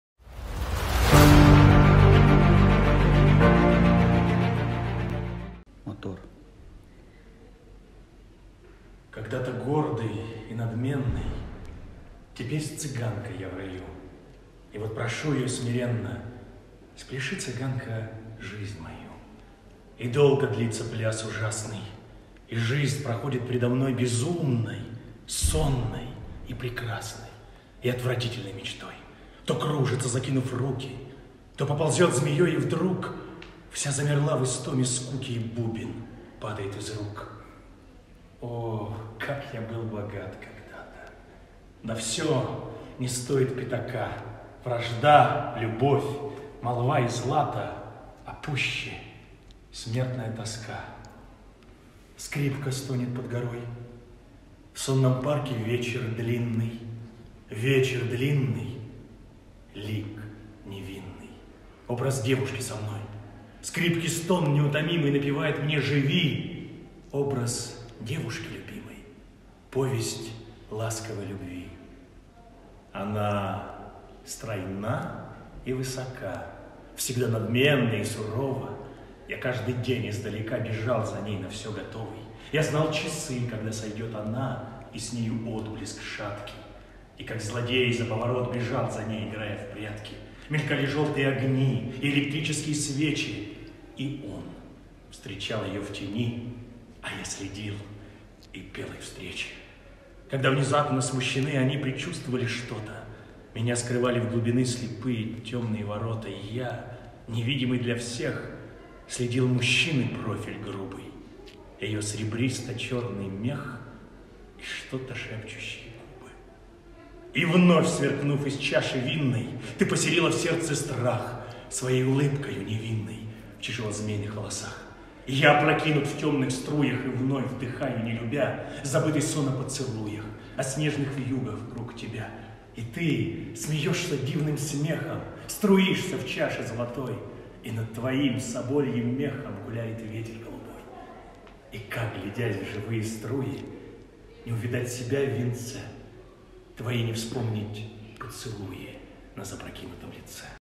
Golden Time Distant Festival | Season 13 | Alexander Milovidov | GTCA-1001-0213
Имя участника или коллектив: Alexander Milovidov
Страна: Russia
Название конкурсной работы: Повесть ласковой любви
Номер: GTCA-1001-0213
Номинация: Искусство слова
Возрастная категория: старше 20 лет
SEASON 13 ♛ CANADA ♛
Участие в других конкурсах и фестивалях/творческие достижения: Дельфийские игры 2008г-золотая медаль; Фестиваль моноспектаклей "Монокль"-победитель